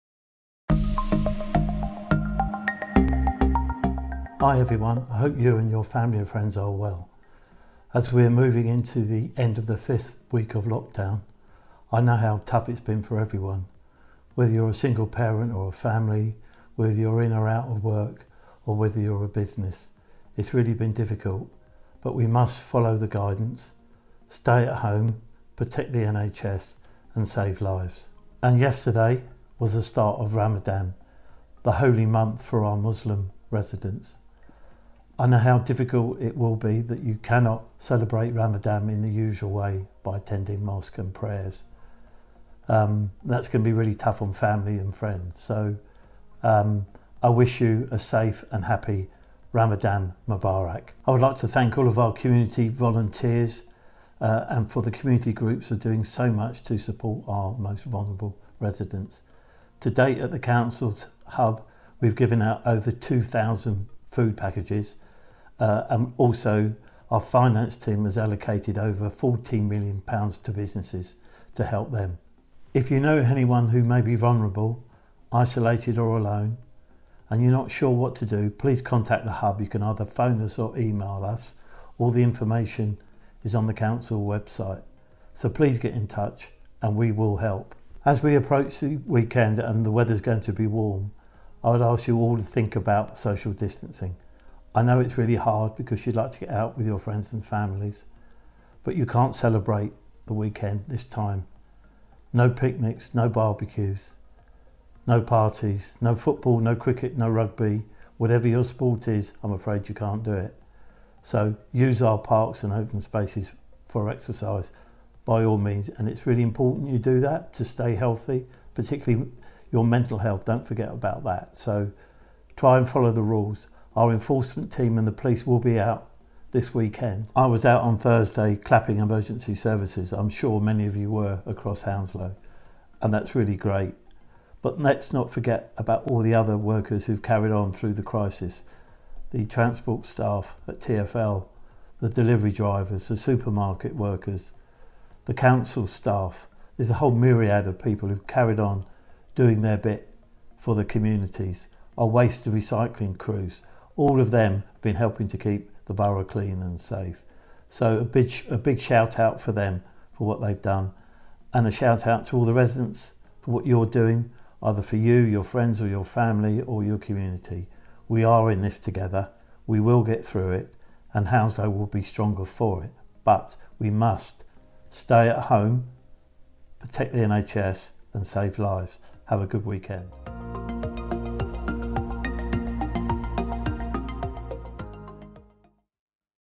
Leader of the Council, Councillor Steve Curran, video message to all residents in Hounslow
Leader of the Council, Councillor Steve Curran, says a few words to you all  before the weekend. Please take a few minutes to watch the video message.